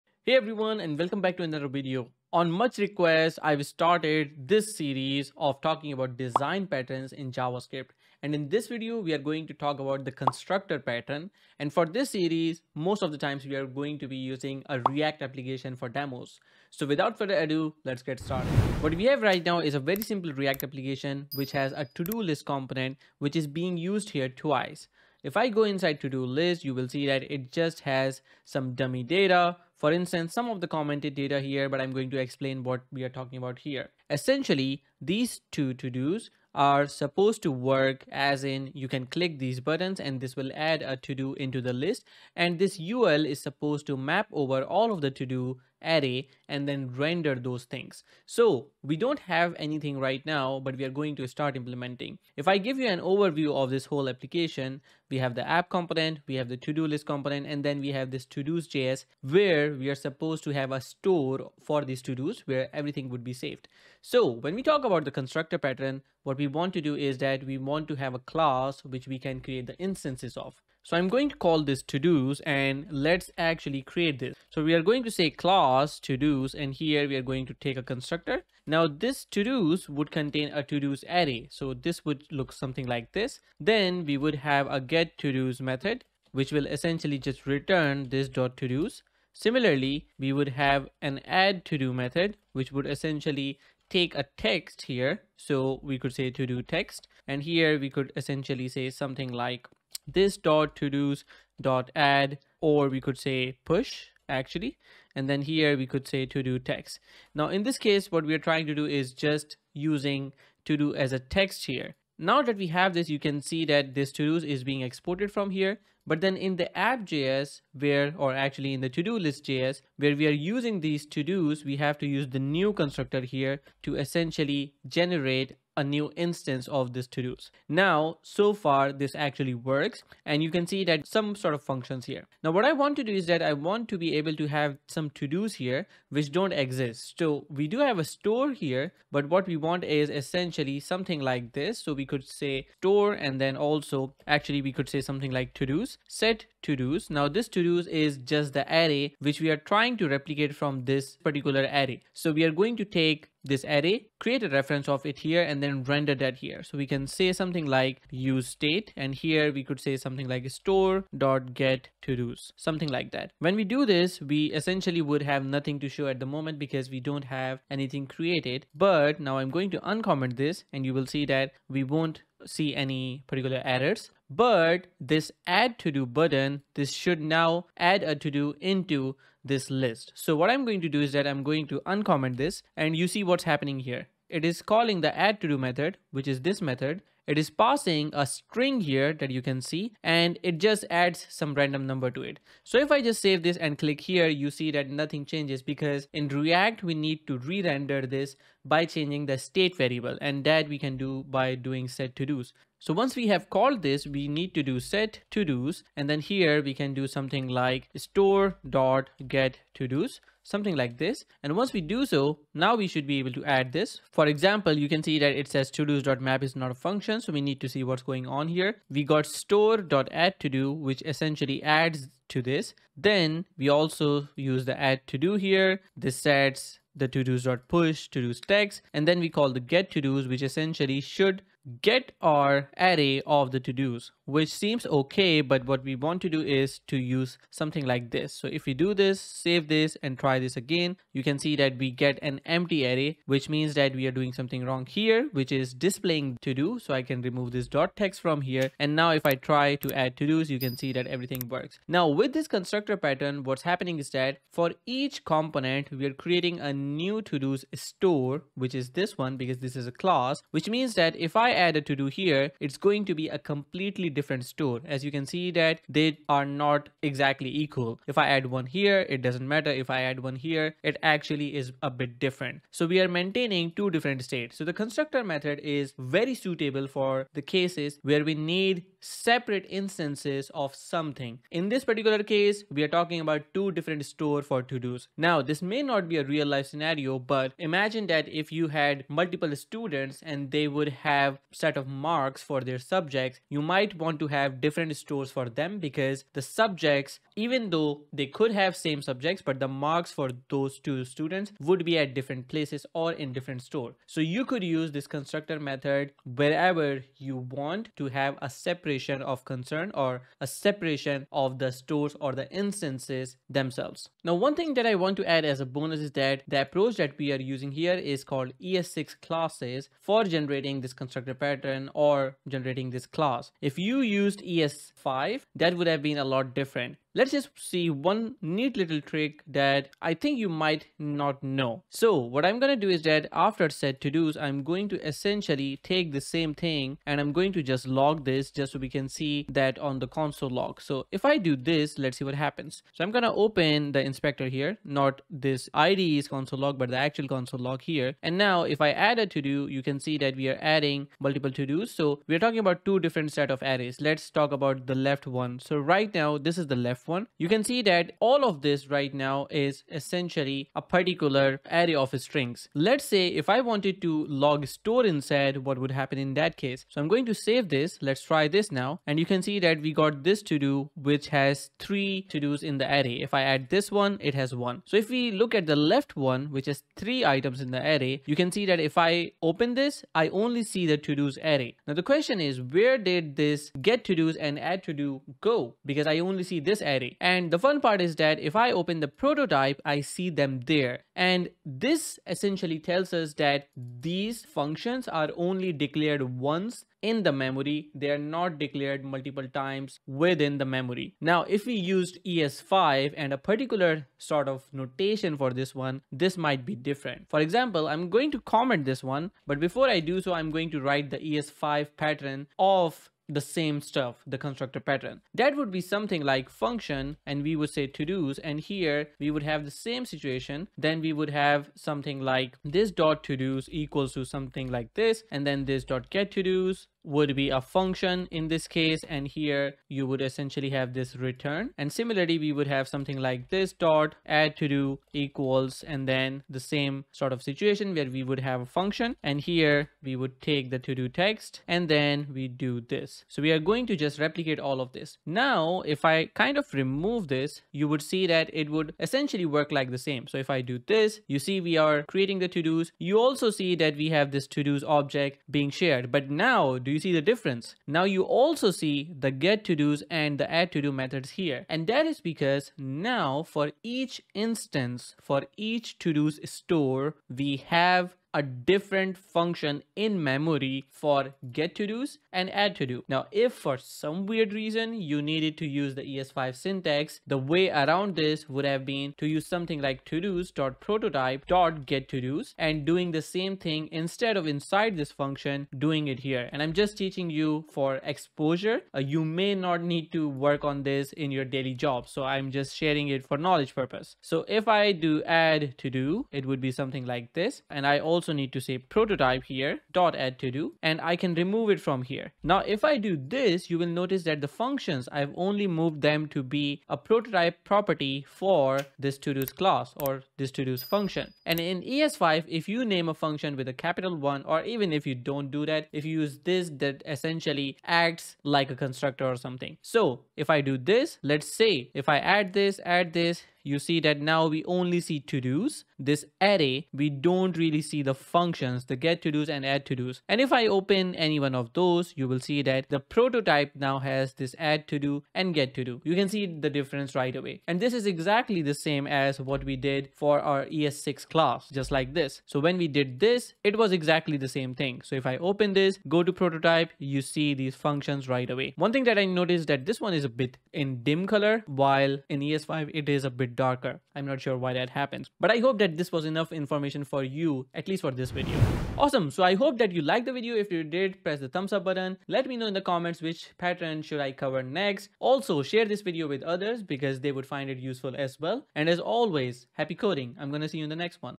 Constructor Pattern in JavaScript
This is a series of videos that revolves around how to work with Design Patterns in JavaScript. In this particular video, we're going to work with the Constructor Pattern in JavaScript. For the purpose of demoing it in a real-world app, we're going to work with a React application. Although, you can essentially use it in any web app with JavaScript/TypeScript.